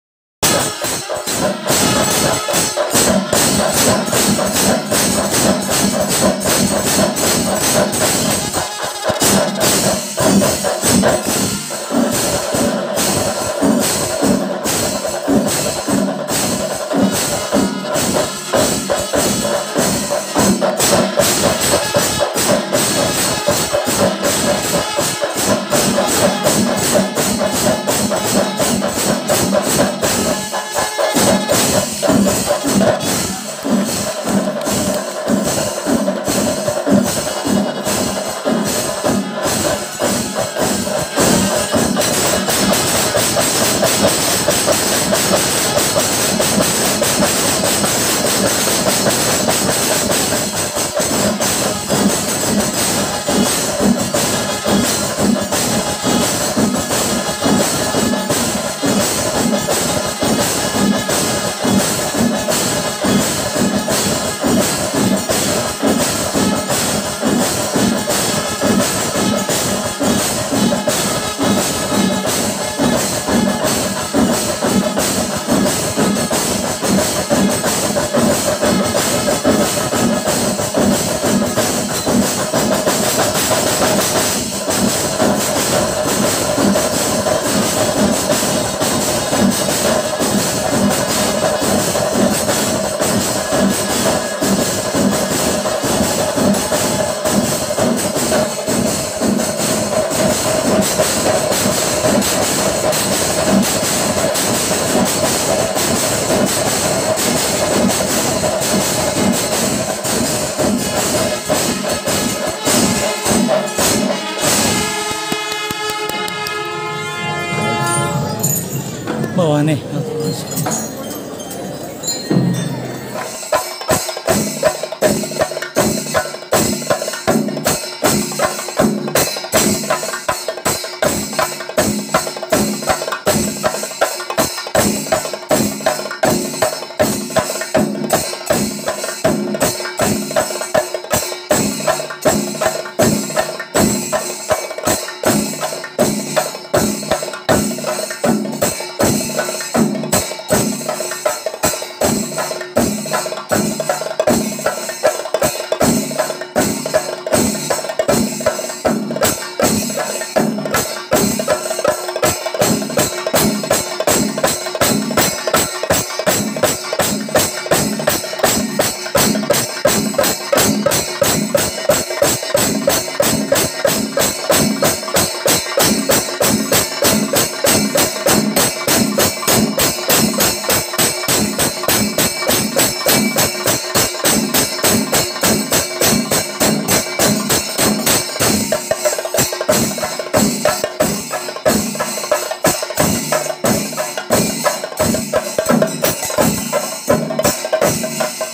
Sree Poornathrayeesa Temple Vrischikolsavam, pancharimelam by Peruvanam Kuttan Marar/ 2022
ശ്രീപൂർണ്ണത്രയീശ ക്ഷേത്രം/ വൃശ്ചികോത്സവംപഞ്ചാരിമേളം 2022/ പെരുവനം കുട്ടൻമാരാർ